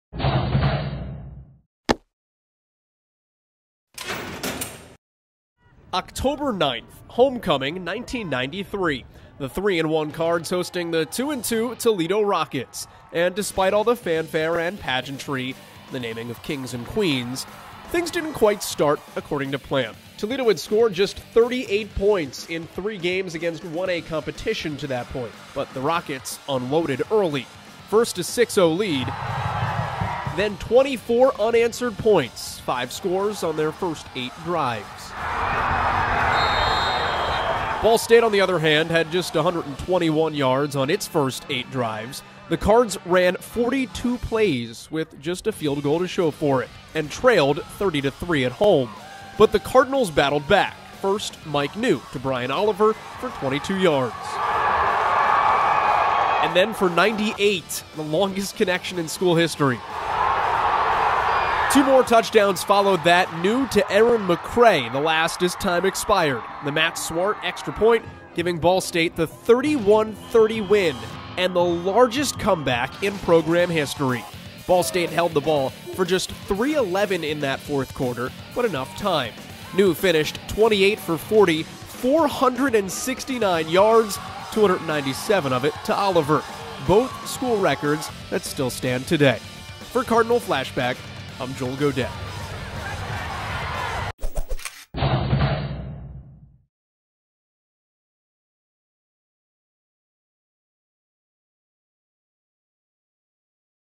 Cardinal Flashback: Football vs. Toledo 1993
Take a look back in the archives as Ball State All-Access reflects on the football team's 1993 Homecoming win over Toledo at Schuemann Stadium.